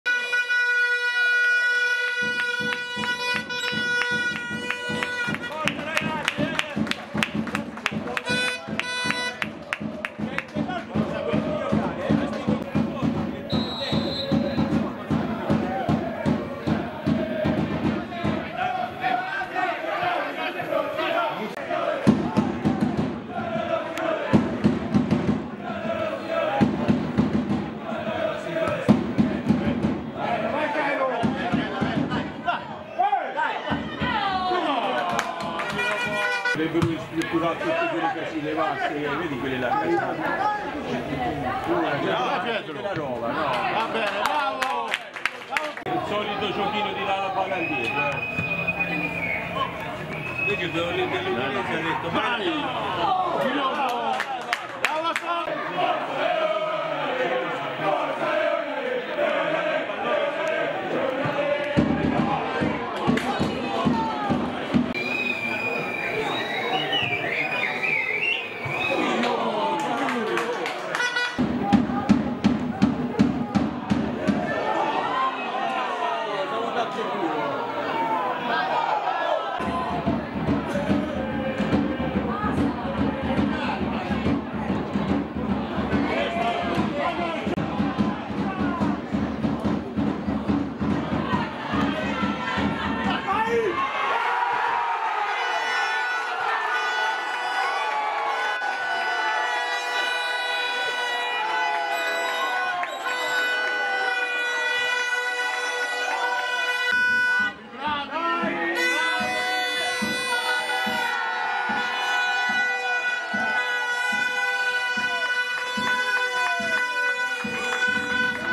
Serie D Girone E Real Forte Querceta-Poggibonsi 1-0
Serie D Girone E Real Forte Querceta-Poggibonsi 1-0, 18 febbraio 2024